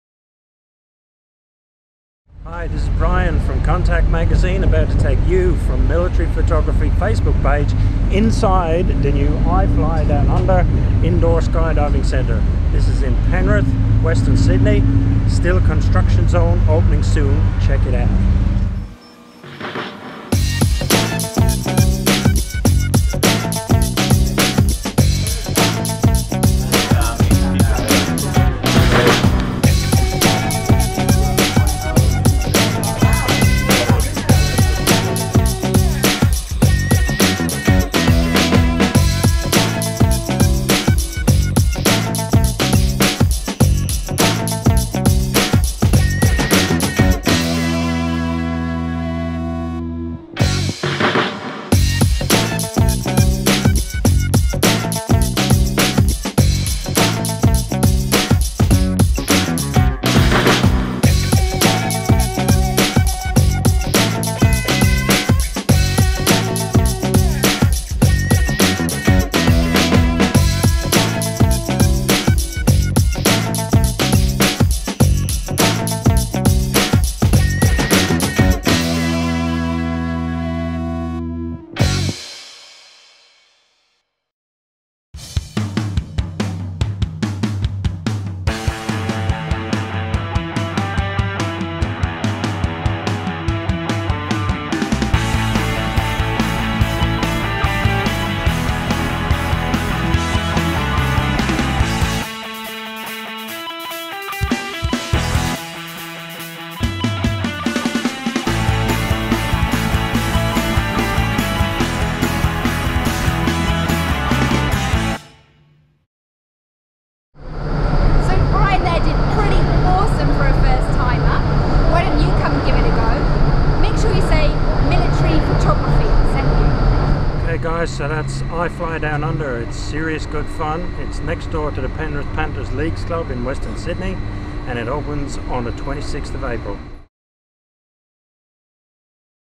iFly Downunder preview flight
iFLY Downunder is Australia's first indoor skydiving centre - and on 16 April, I gave it a test flight, reporting for CONTACT magazine and Military Photography Facebook page.

The official web site says, "iFLY Downunder is a state-of-the-art vertical wind tunnel that provides a fun and safe way to experience true human bodyflight. We fly people from ages 3 to 103. No experience necessary and all training is included."

I say - it is seriously good fun, but is probably not an 'entertainment experience', in that it is quite expensive and end WAAAAY TOO SOON! Time literally does fly when you're having fun!

But, as a serious indoor sports-training facility, it simply cannot be beaten. Consider that a standard skydive might last 60 to 90 seconds in the free-fall stage. But with iFly, individuals or groups can train/rehearse their free-fall techniques and manoeuvres in the comfort and safety of an indoor facility for an hour or more at a time.

It simply can't be beaten as a training facility.

The iFly Downunder facility, next door to the Penrith Panthers Leagues Club on Mulgoa Rd, Penrith, is still in the last throes of construction, but will be open to the public on 26 April 2014. for details and bookings.

In the mean time, check out my first ever 'flight' in this video.

Brian Hartigan
Managing Editor
CONTACT Air Land & Sea plus
COMBAT Camera magazines